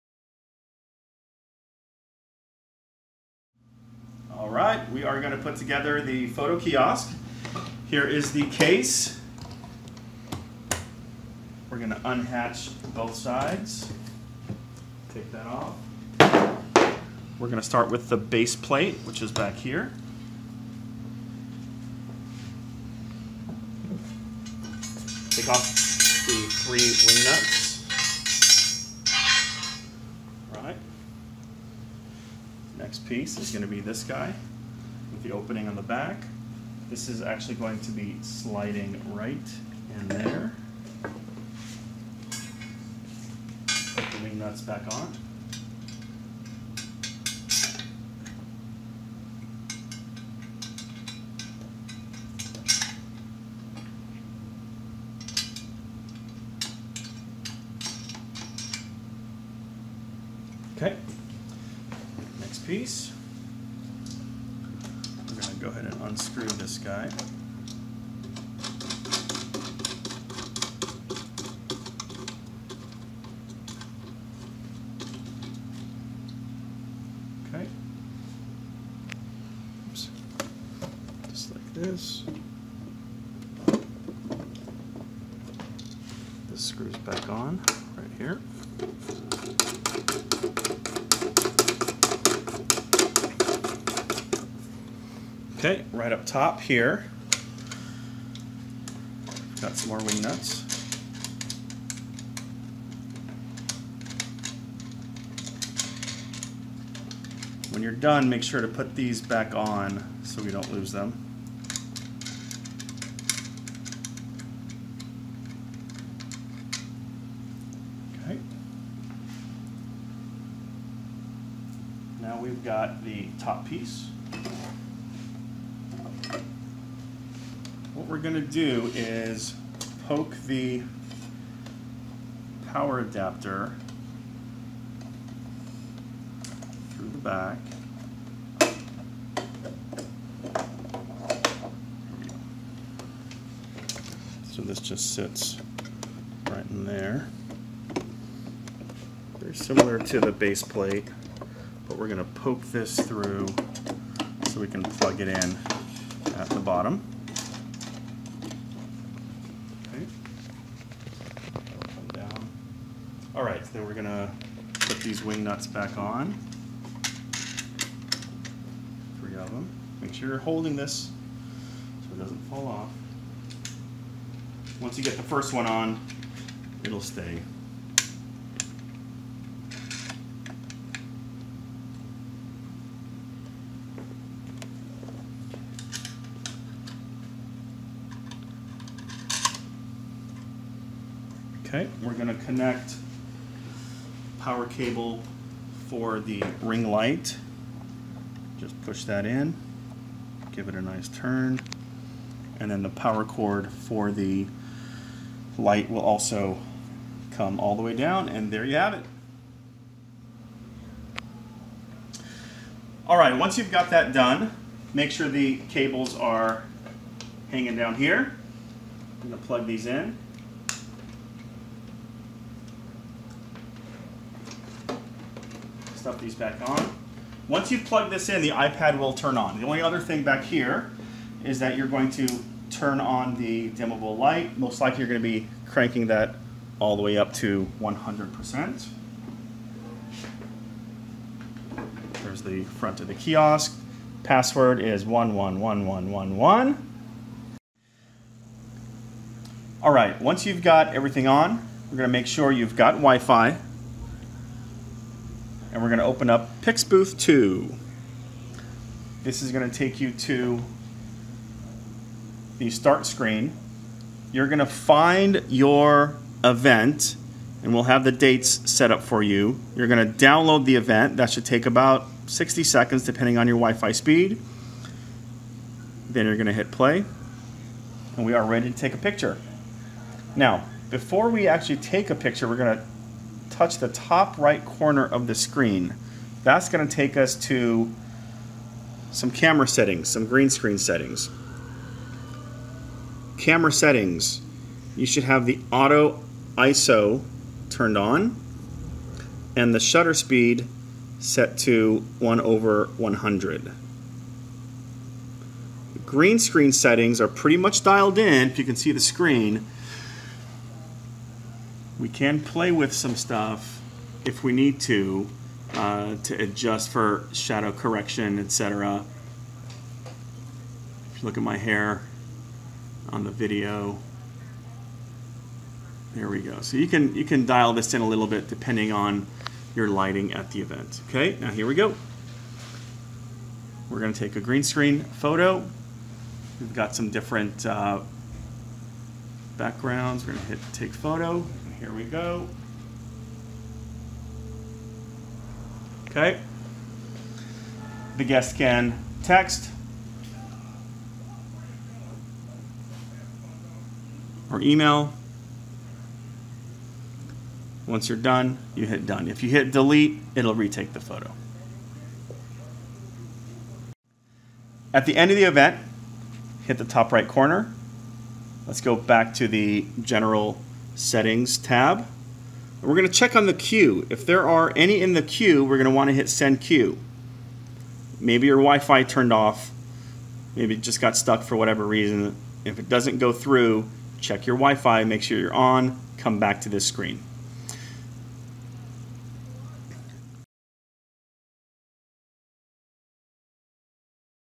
Digital Photo Kiosk Tutorial Video
A quick tutorial covering kiosk assembly and using the photo app.